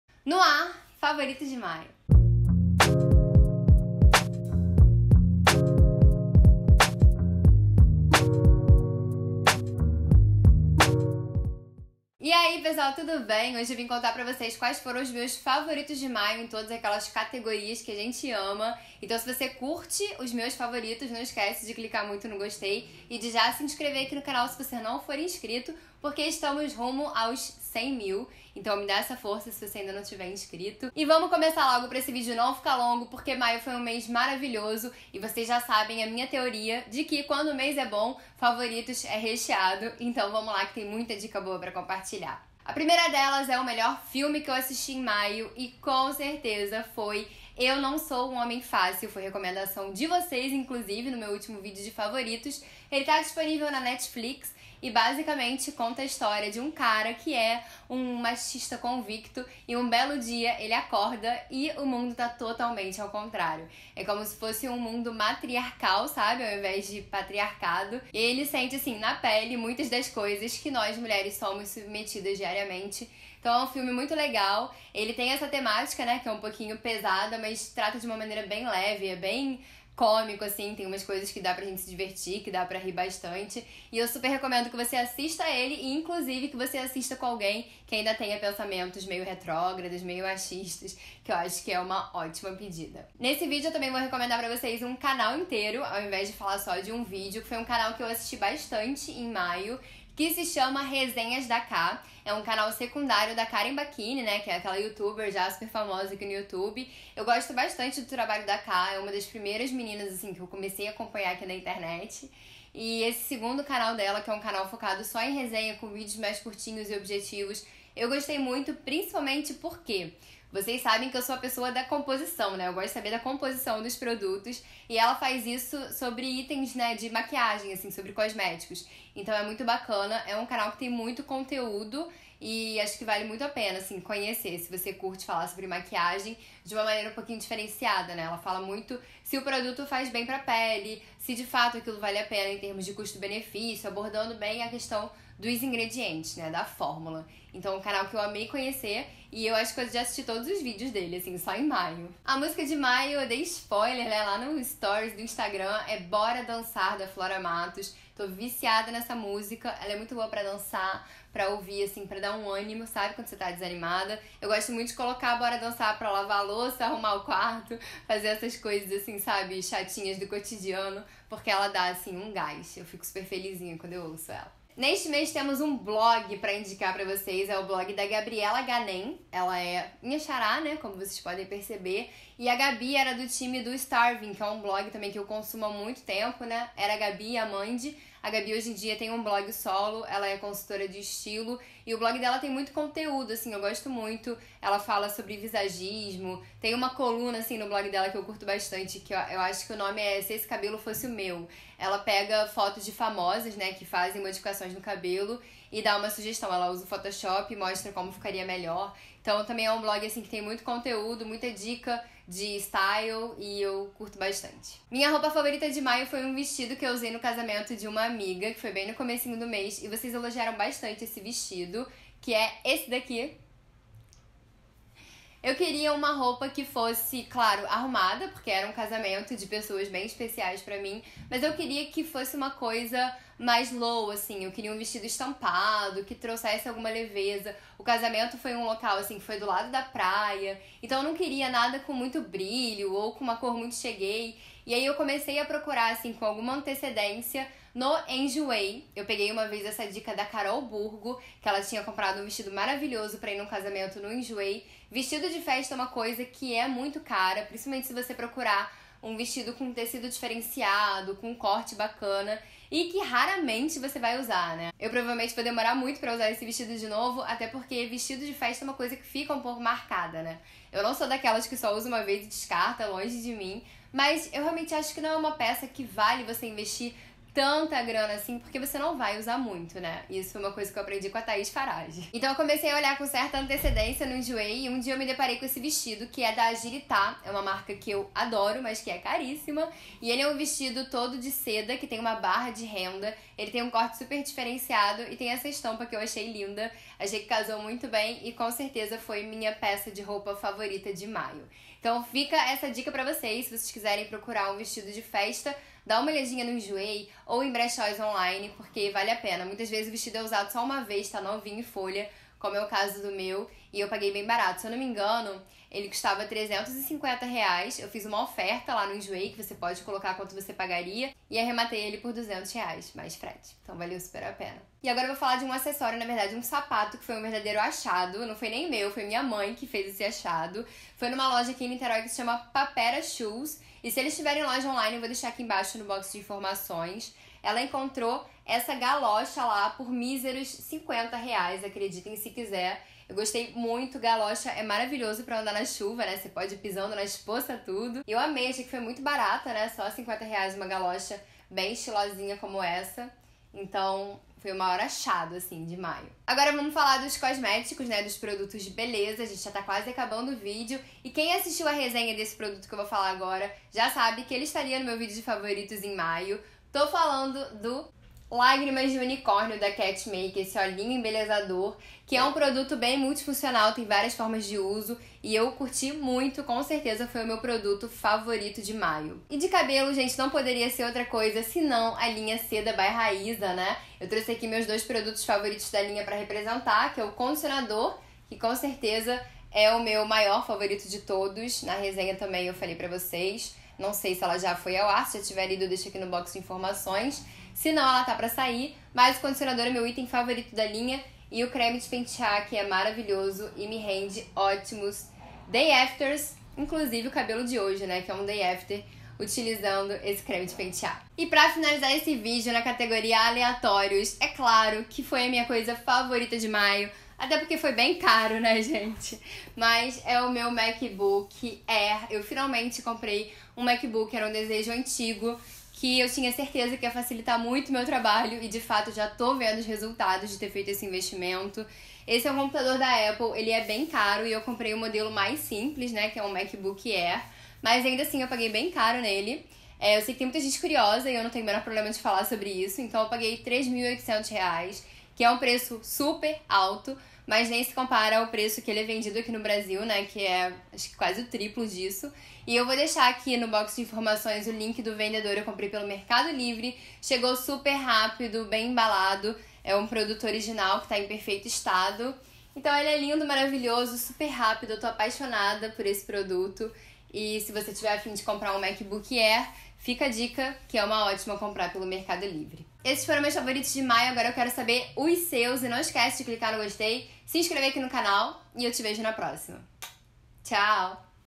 FAVORITOS DE MAIO!
FAVORITOS DE MAIO NO AR! Espero muito que gostem! ♥
* Assista em HD e expanda para mais informações! *
- Melhor filme: Eu não sou um homem fácil - disponível na Netlix
- Melhores cosméticos: Lágrimas de Unicórnio Catmake, Seda by Rayza